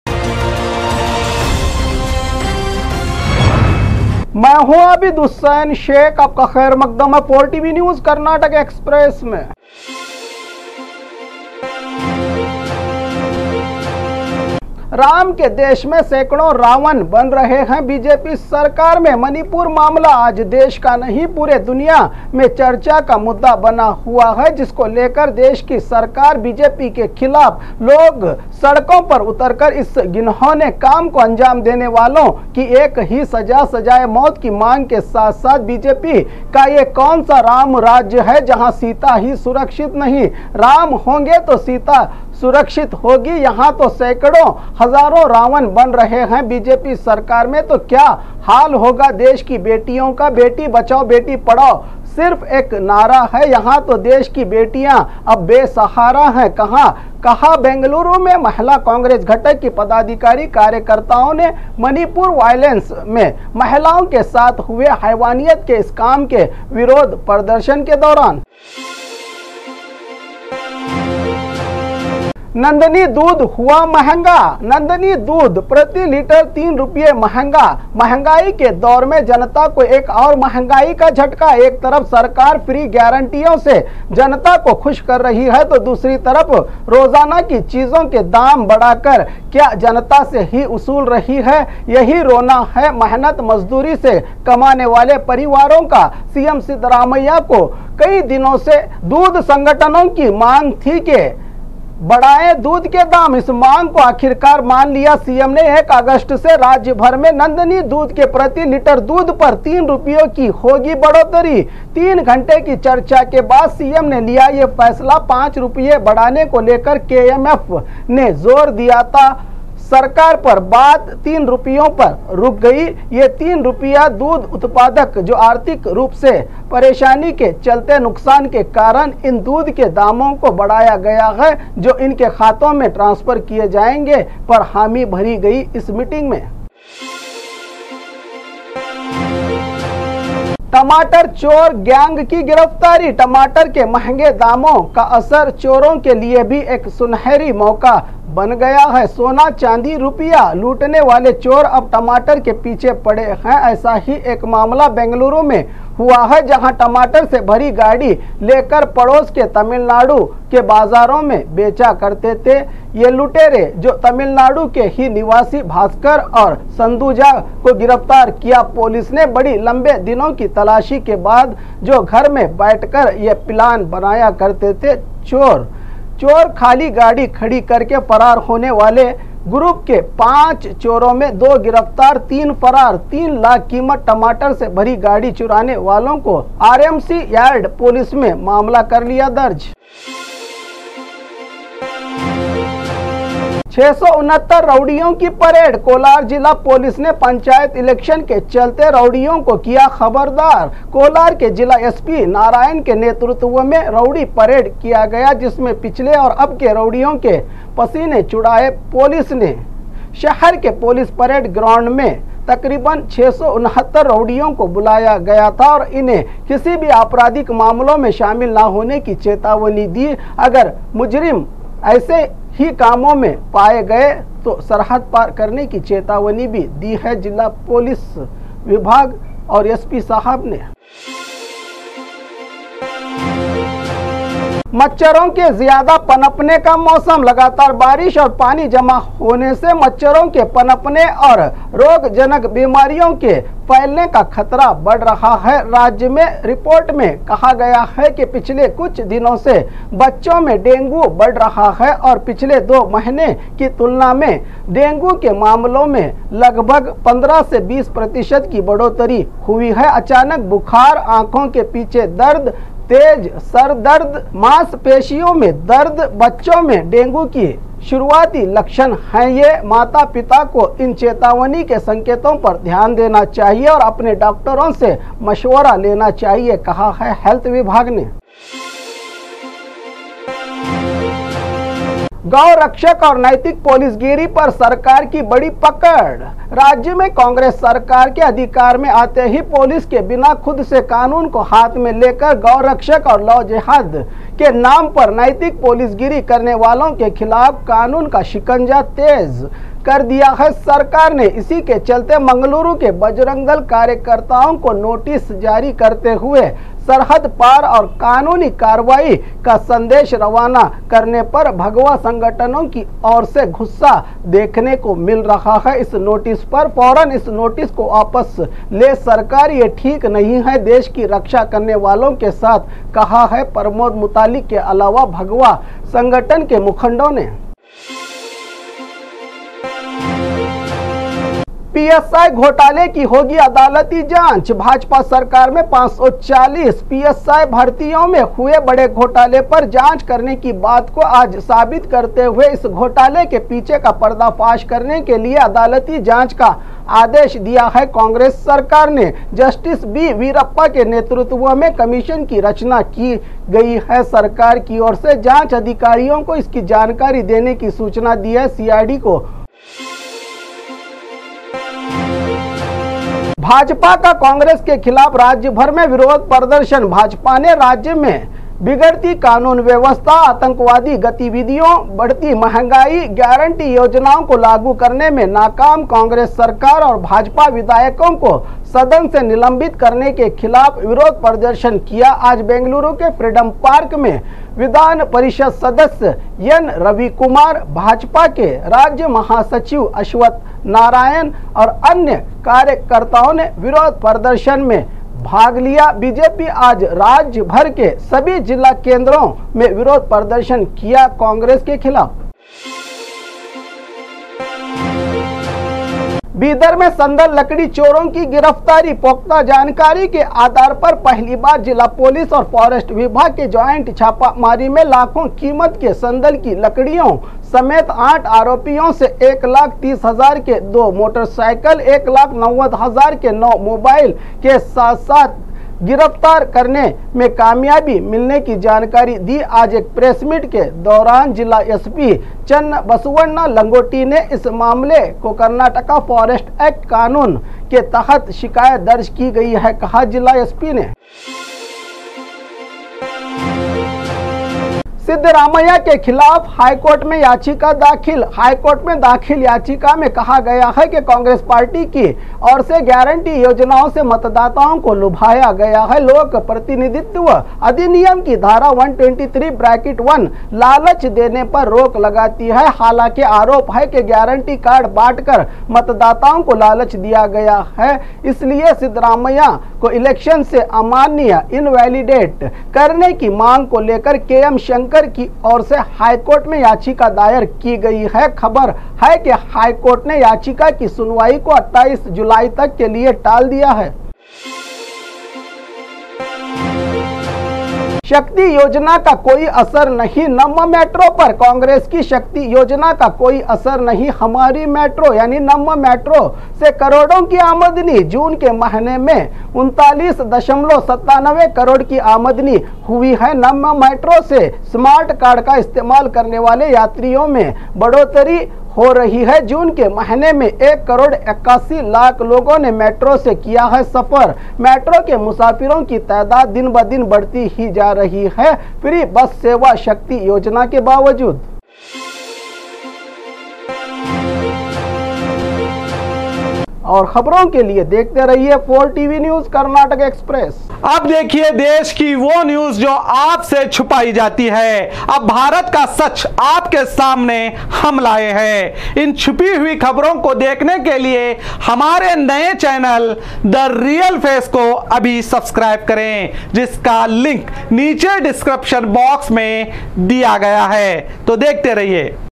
4 TV News Karnataka Express | कर्नाटक एक्सप्रेस | 22-07-2023
4 TV News Karnataka Express | कर्नाटक एक्सप्रेस |
1; Ram ke Desh mein saikdon Ravan banaa rahi hai BJP
Manipur mamla Aaj Desh ka nahin pure duniya mein charcha ka Mudda banaa hua hai jisko lekar desh ki sarkar BJP ke khilaf log sadkon per utar kar is ghinaune kam ko anjam dene walon ki ek hi Saja sajayen maut ki mang ke sath sath BJP ka ye kaun sa Ram rajya hai Jahan Sita hi surakshit nahin Ram honge to Sita surakshit hogi yahan to saikdon hajaron Ravan ban rahe  BJP sarkar  to kya hal hoga desh ki betiyon ka Beti bachao beti padhao sirf ek Nara hai yahan to desh ki betiyan besahara hai  kahan Bengaluru mein mahila Congress Ghatak ki padadhikari karykartaon ne Manipur violence mein mahilaon ke sath huve haivaitat ke is kaam ke virodh pradarshan ke dauran

2; Nandani dudh huva mahanga
Nandini dudh prati litre ₹3 mahanga mahangai ke Daur mein Janata Ko ek aur mahangai ka jhataka ek taraf Sarkar free guaranteeyon se Janata ko Khush kar rahi hai to dusri taraf rojana ki chijon ke dam badhakar kya Janata se hi usul rahi hai yahi Rona hai mehnat majduri se kamane wale parivaron ka CM siddramaiya ko Kai dinon se dudh Sangatano ki mang thi ke badaye  dudh ke daam is mang Ko aakhirkar man liya CM ne 1 August se rajya bhar mein Nandini dudh ke prati litre dudh per teen rupaye ki hogi badhotari 3 ghante ki charcha ke bad CM ne liya ye faisla 5 rupaye badhane ko lekar KMF ne Jor diya tha Sarkar per baat ₹3 rupayon per ruk gai yah teen rupya dudh utpadak Jo arthik roop se pareshani ke chalte nuksan ke Karan in dudh ke Damon ko badhaya gaya hai jo inke khaton mein transfer Kiye jayega per hami Bhari gai is meeting mein

3;Tamatar chor Gyan ki girftari
tamatar ke mahange Damon ka asar choron ke liye bhi ek sunhari mauka ban gaya hai Sona Chandi rupya lootane wale chor ab tamatar ke piche pade Hain Aisa hi ek mamla Bengaluru mein hua hai Jahan tamatar se Bhari gadi lekar pados ke Tamilnadu ke bajaron mein becha karte the yah lutere Jo Tamilnadu ke hi nivasi Bhaskar aur sanduja Ko girftar Kiya  police ne badi lambe  dinon ki talashi ke bad jo ghar mein baithkar yah plan banaya karte the chor Khali gadi khadi karke farar hone wale group ke panch choron mein do  girftar teen farar teen lakh kimat tamatar se Bhari gadi churane walon Ko RMC yardd police me mamla kar liya hai darj

4;669 roudiyon ki parade
Kolar jila police ne panchayat election ke chalte roudiyon Ko kiya khabardar  Kolar ke  jila SP  Narayan ke netrutva mein raudi parade Kiya Gaya jismein pichhle aur abke roudiyon ke pasine chhudayen police ne Shahar ke police parade ground mein takriban 669 roudiyon Ko Bulaya gaya tha aur inhe kisi bhi apraadhik mamalon mein Shamil na hone ki chetavni Di agar mujrim aise kamon mein paye Gaye to sarhad paar karne ki chetavri bhi di police vibhag ne

5; Machharon ke jyada panpane ka Mausam
lagatar barish aur Pani jama hone se machharon ke Panpne aur rogjanak bimariyon ke failne ka khatra badh Raha hai rajya mein report mein kaha gaya hai ki pichhle kuchh dinon se bacchon mein dengu badh Raha hai aur pichhle do mahinon ki tulna mein dengu ke mamalon mein lagbhag 15 se 20% ki badhotari Hui hai Achanak bukhar aankhon ke piche Dard Tej sar Dard manspeshiyon mein Dard bacchon mein dengu ki shuruaati lakshan hai ye Mata pita ko  in chetavni ke sanketon per Dhyan Dena chahiye aur apne doctoron se mashvara Lena chahie kahan hai helth vibhag ne